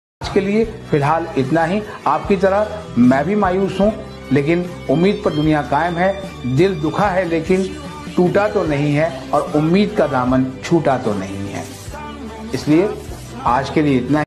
Dil Dukha Hai Lekin Tuta To Nahi Meme Template
"Dil Dukha Hai, Lekin Tuta To Nahi" meme is going viral on social media. Here's the full meme template that you can download and use to create your own memes online.

"Dil Dukha Hai Lekin Tuta To Nahi Meme Template" is a must-watch event that you won't want to miss.